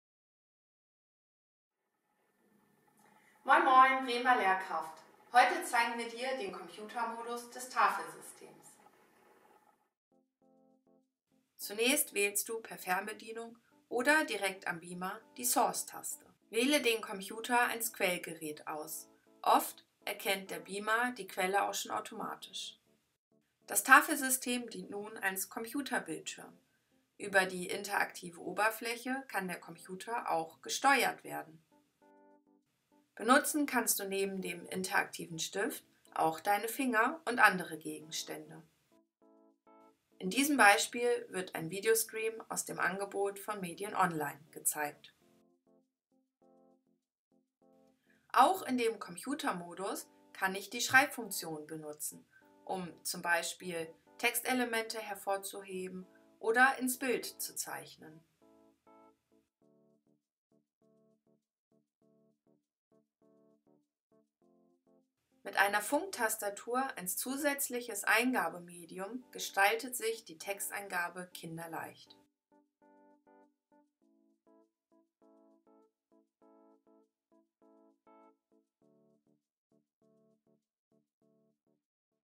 Das interaktive Tafelsystem: der Computermodus
Diese Video zeigt den Computermodus der Interaktiven Tafel.